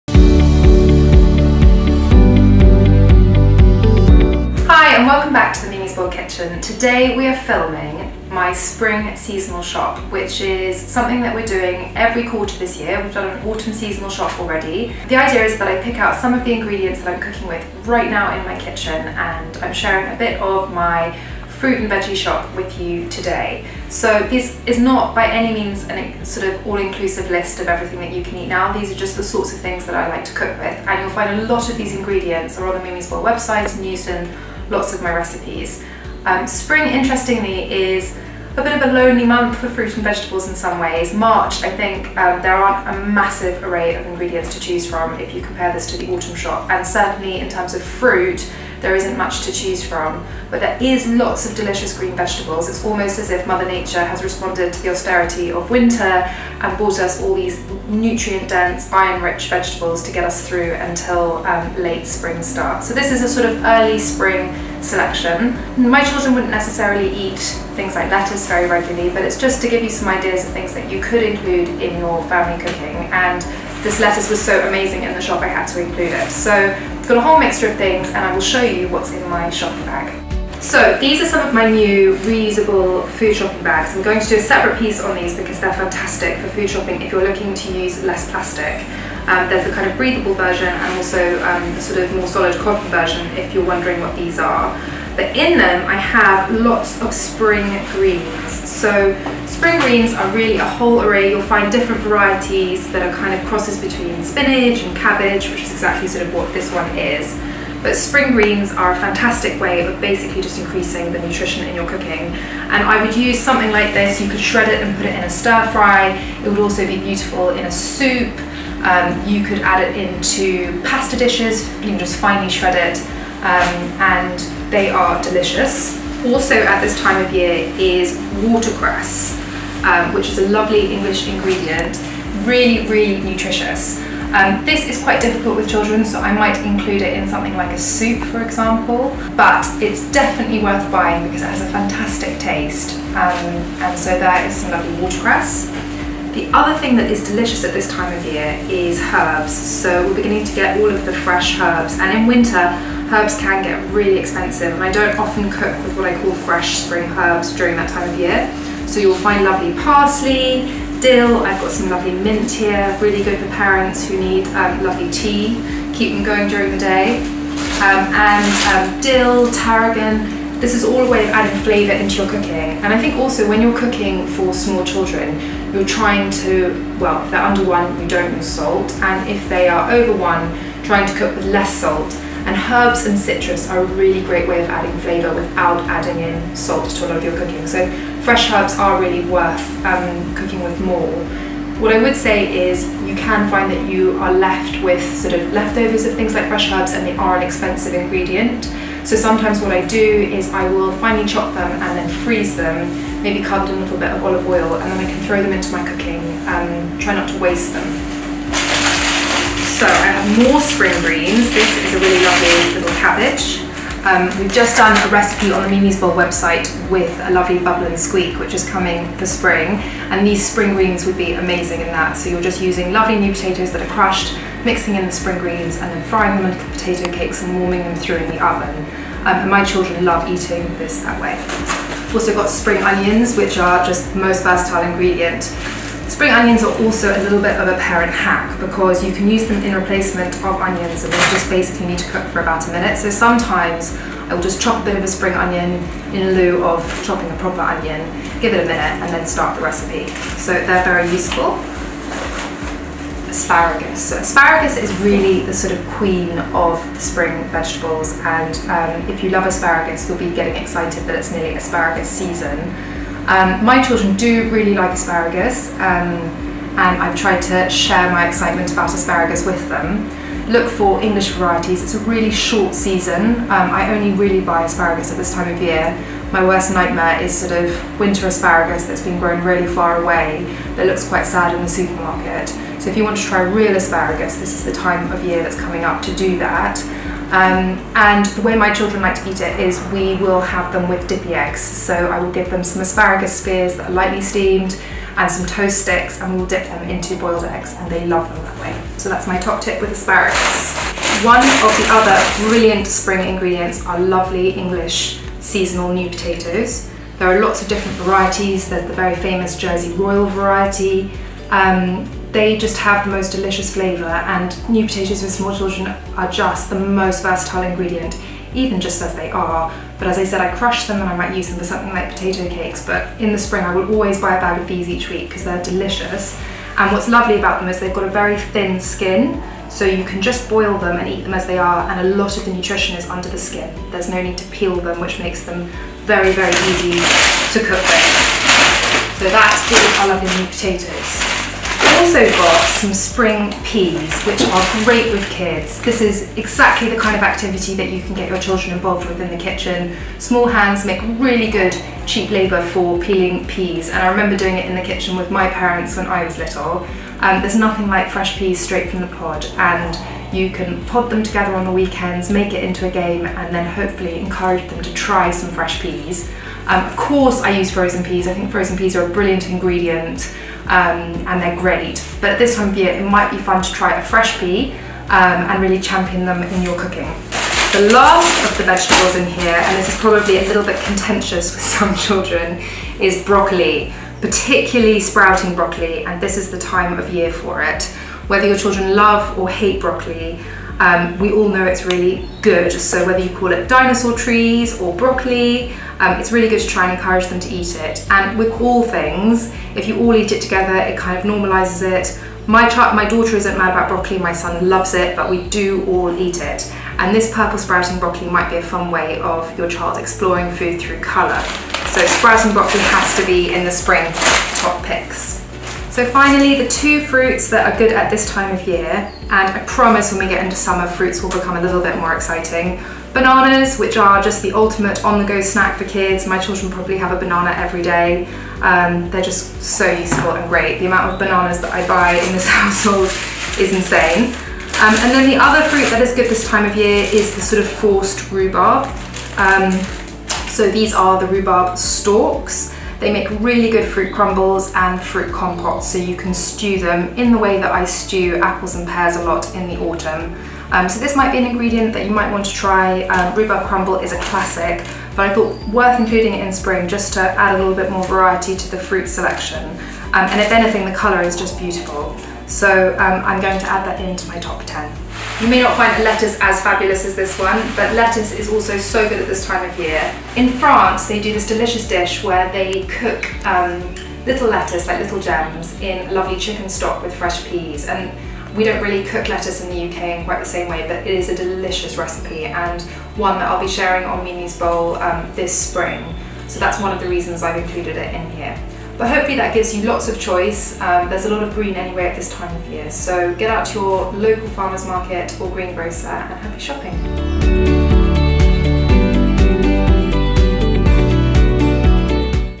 Spring's Seasonal Food Shop
Join me Miriam in the mimisbowlkitchen for my top pick of fruits and vegetables, in early Spring.

March is one of the slowest month's for Seasonal food in the UK. The austerity of Winter is passing; the first signs of Spring just appearing, and yet UK-grown produce isn't in full swing. So this video is to help you find the best, of what there is to enjoy and eat!

As a parent it can be difficult to feel inspired by getting dinner on the table and these seasonal shops hope to inspire new ingredients and recipe ideas. In the bigger picture, teaching our children about eating with the seasons; in an age, where we can eat anything we want, at any time of year seems crucial. I hope they’ll be the generation to teach us better ways.

Mimi's Bowl: The Spring Shopping List
Vegetables ~ spring greens, cabbage, sprouting broccoli, spring onions, new potatoes, the first asparagus, green house lettuces and herbs
Fruits ~ (forced) rhubarb